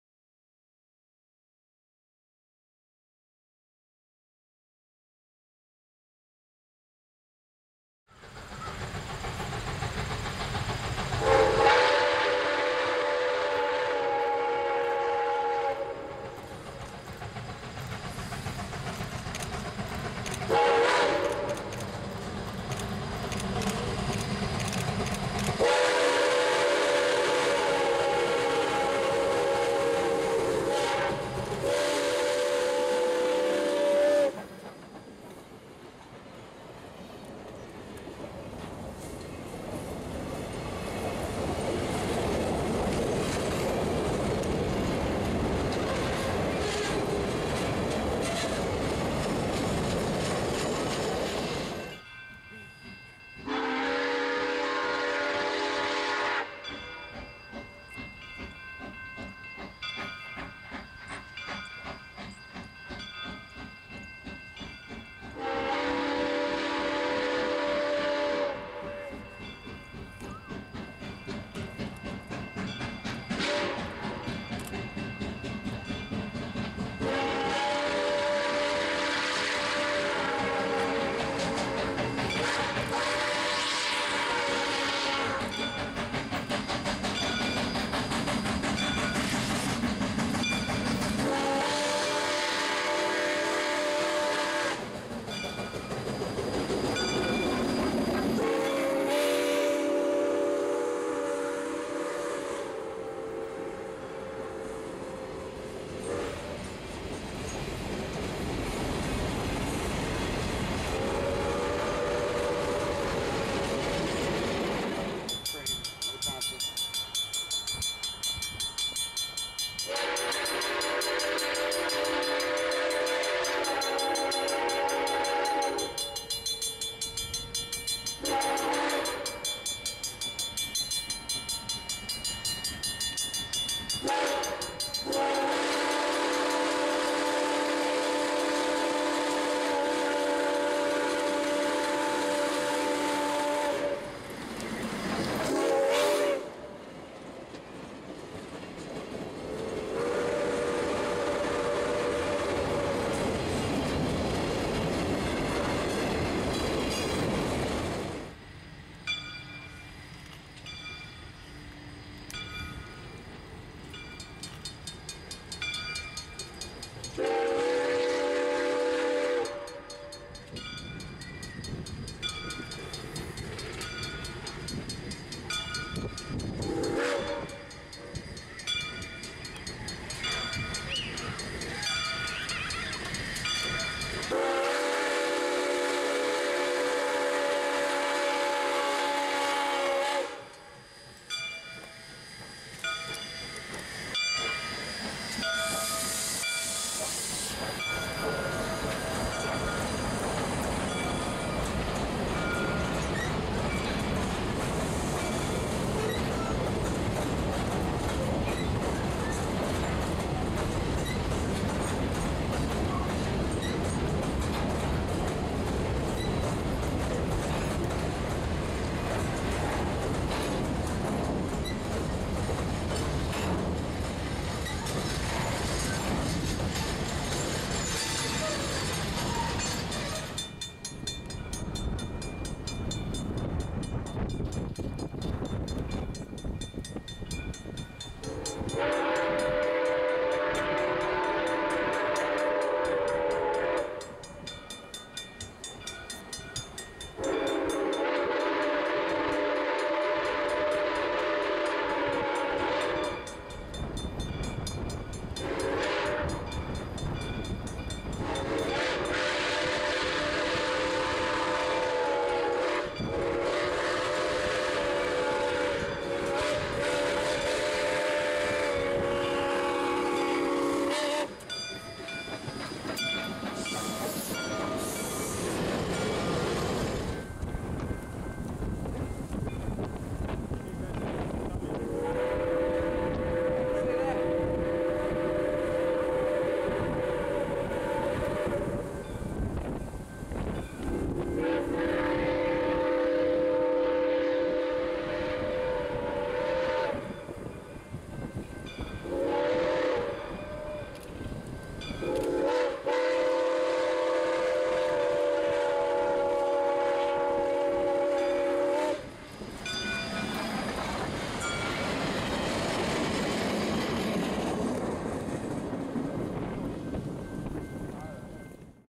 SOO 1003 East on the WSOR Cambria and Milwaukee Subs 10/23/16
On the weekend of October 22-23, SOO 2-8-2 Mikado-Type #1003 made a "private" photo run on the WSOR Northern Division between Hartford and Fox Lake Junction. On the last day of the run, I chased it from Fox Lake Junction to Iron Ridge with stops at Beaver Dam, Minnesota Junction, and Horicon.